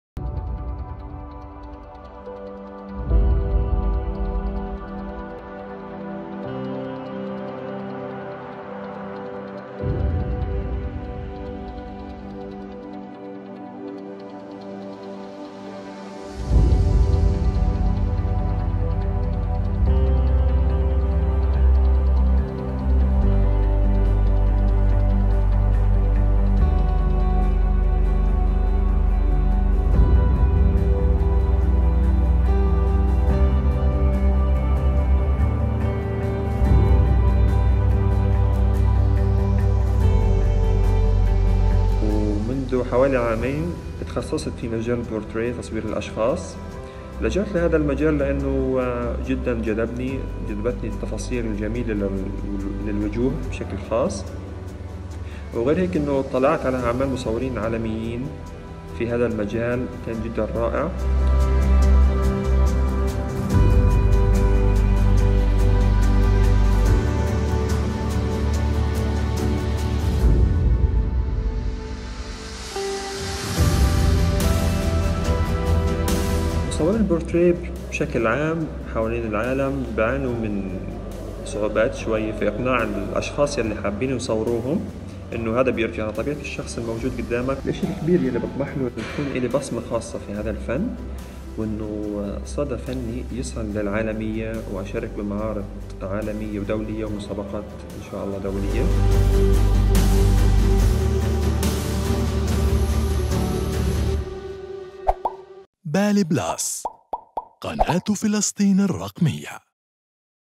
شاهد مصور فوتغرافي فلسطيني يبدع في صناعة الصورة النابضة
التقط بعدسة الكاميرا أجمل اللوحات الفنية وأبدع في صنع الصورة النابضة بالحياة، تعرف عليه.